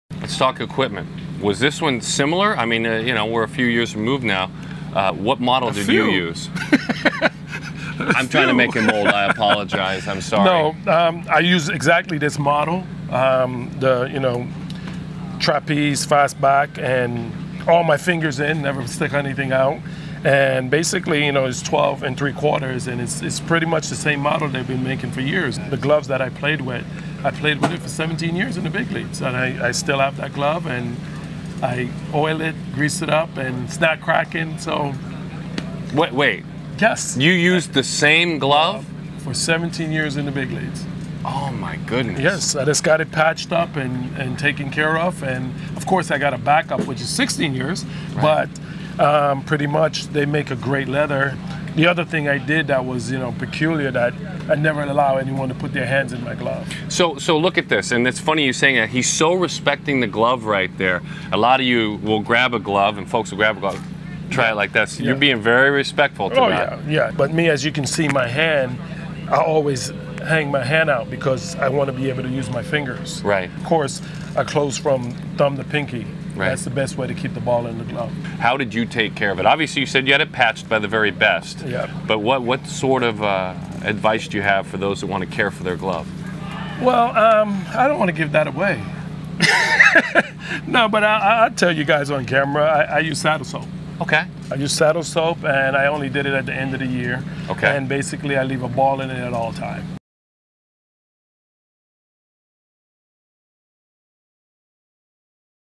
Devon White and his Rawlings Glove
Devon White might be a 7x Gold Glove winner, but he only actually used one glove on the field for his entire career. He talks to Daron Sutton about how he kept his Rawlings leather in perfect condition for 17 years.